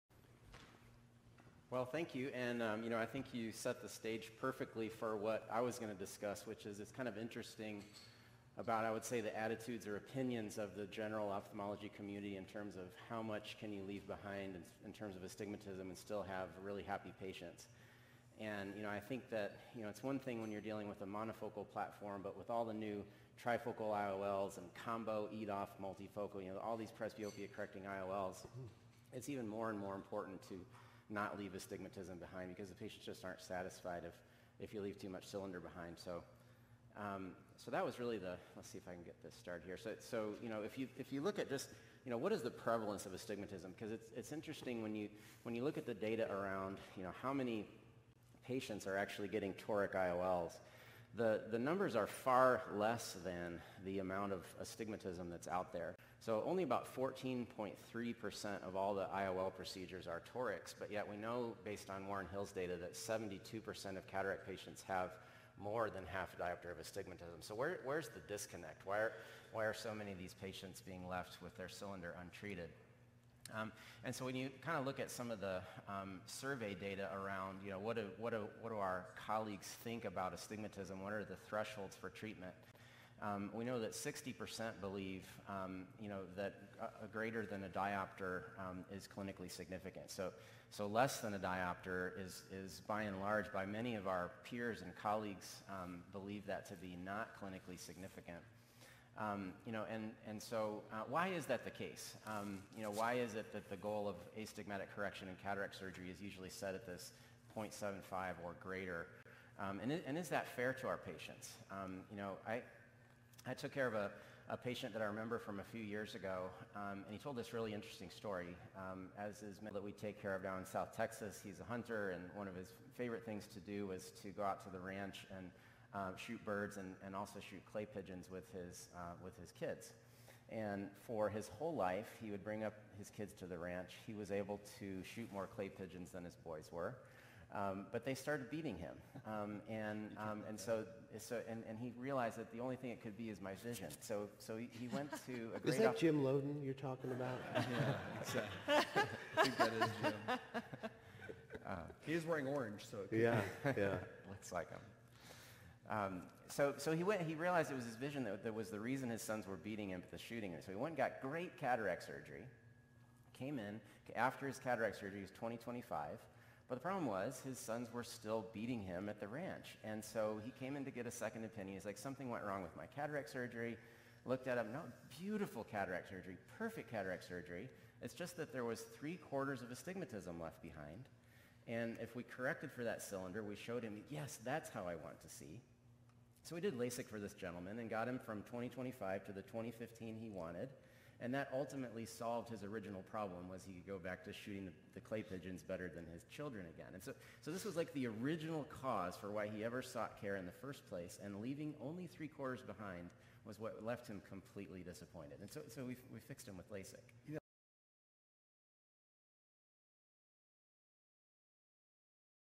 Dr. Parkhurst Shares the Importance of a Refractive Mindset
Dr. Parkhurst asks the question: How much astigmatism can you leave behind and still have happy patients? ✎ Share your experience in the comments.

1.    Market Scope. 2021 IOL Market Report: A Global Analysis for 2020 – 2026
2.    Dr. Warren Hill. Distribution of Corneal Astigmatism – Normal Adult Population. Accessed August 22, 2017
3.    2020 CRST Clinical Survey of 450 cataract and refractive surgeons by The Fundingsland Group. October 2020.